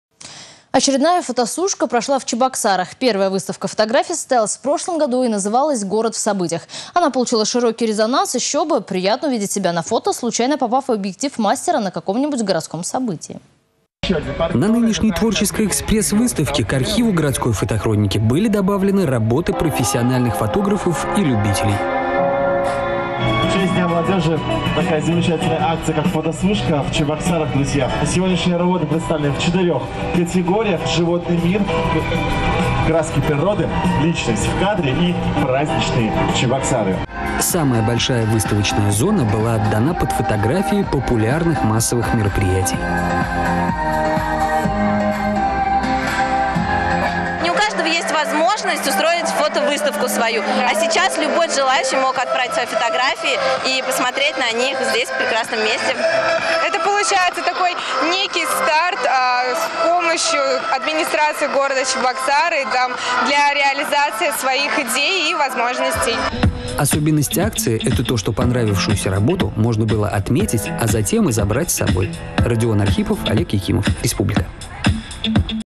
Очередная фотосушка прошла в Чебоксарах
Первая выставка фотографий состоялась  в прошлом году  и  называлась «Город в событиях». Она  получила широкий резонанс. Ещё бы, приятно увидеть себя на фото, случайно попав в объектив мастера на каком-нибудь городском событии.